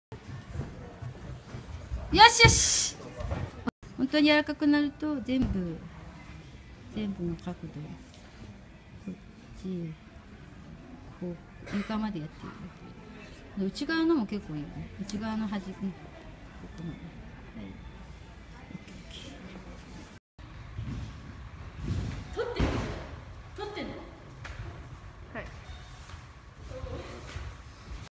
BODY WORK by eico hatta
体操競技のBODY WORKの成果。

・高速くるくる〜　よしよしっ！
 感覚を植え付けるだけで回れますよっ。

・セルフケアを徹底して、1年半の成果、男子でもここまで柔らかくなる〜
　コアもついて、いい感じでパフォーマンス力アップできています。怪我も激減です。
　背中の関西魂がまぶしいぜっ！笑

・自分の成果。愛知の北折先生の新しいスタジオで、競技用のトランポリンで飛ばせてもらいました。
まだこのくらいはできた〜〜。楽しい〜〜！